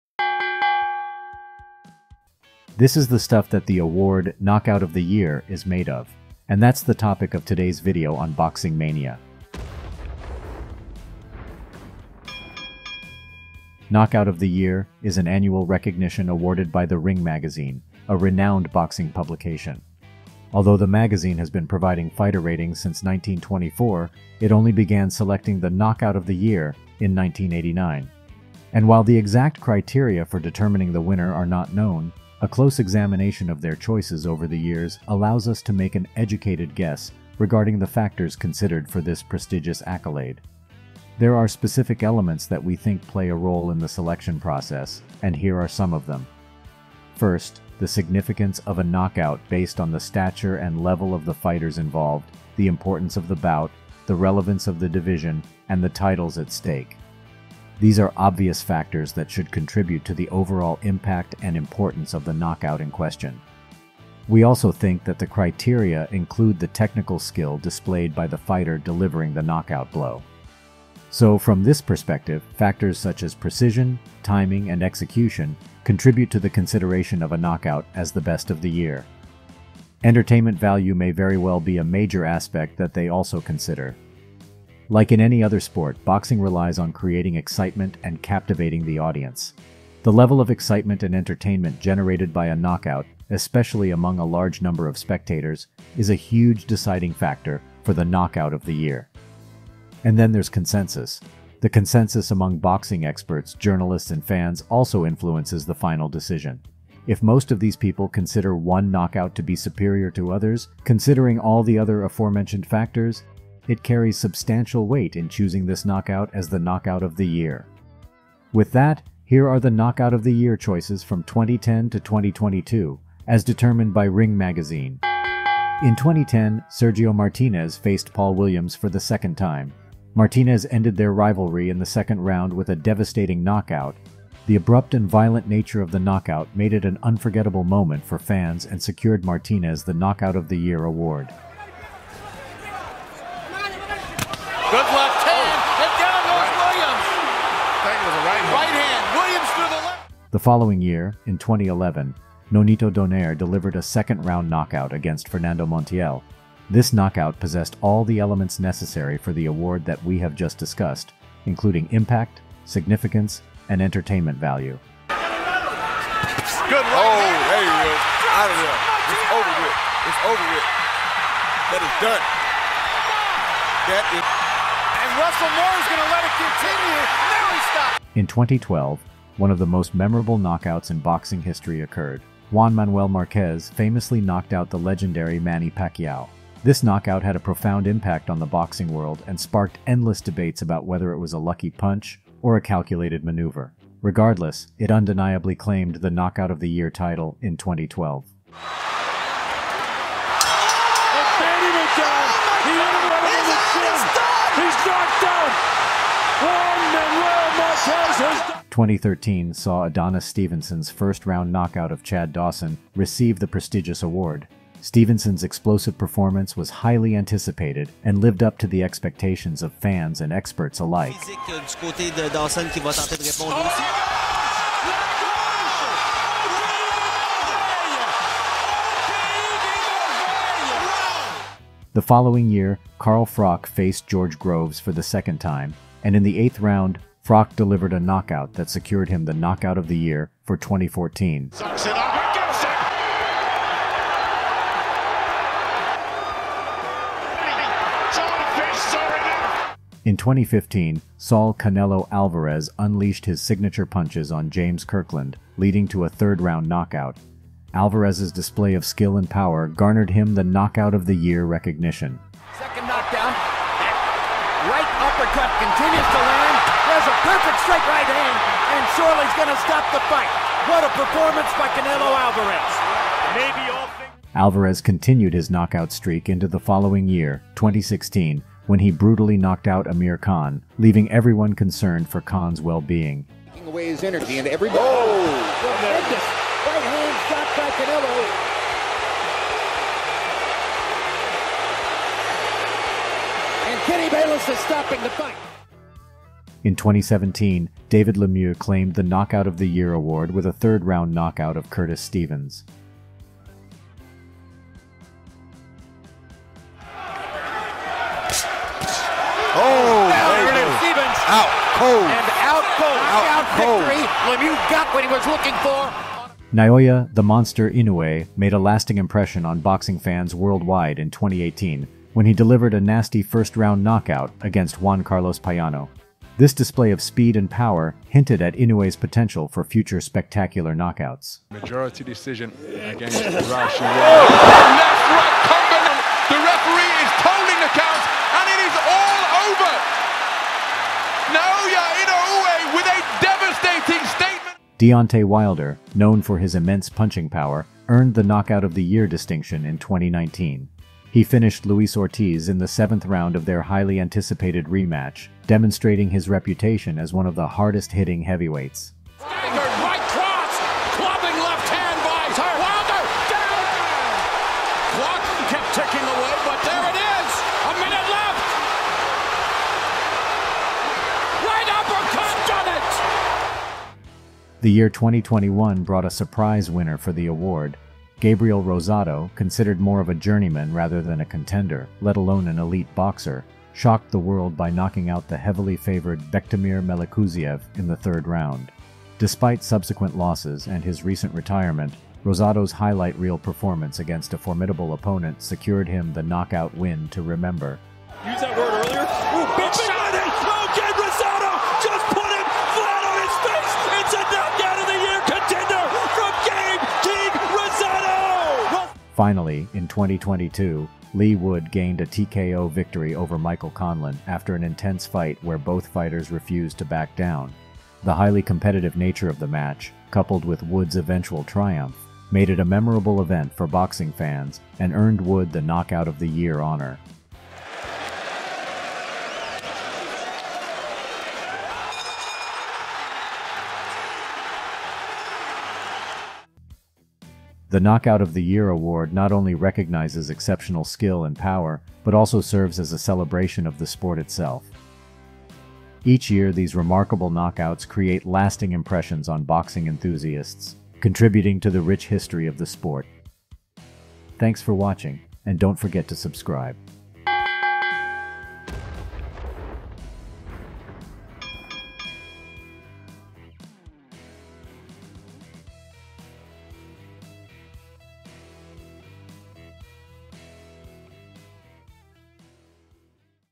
How Does a KO Become "Knockout of the Year?" | KO of The Year 2010-2022, & Everyone's Bet for 2023
What is the Knockout of the Year?: 0:53
How is the 'Official' Knockout of the Year Chosen?: 1:20
Knockout of the Year Awardees from 2010 to 2022:  2:37

What do people instinctively blurt out whenever they witness a stunning stoppage in the ring?

"This is the knockout of the year!"

That's right, and it's not difficult to see why.

But not every sports fan is aware that Knockout of the Year is an actual, legitimate, and well-regarded award in boxing.

In other words, although anyone can have a personal choice or opinion on the matter, there is an official award that is recognized by the larger boxing community.

This video explains this, and also presents the Knockout of the Year awardees of 2010 to 2022.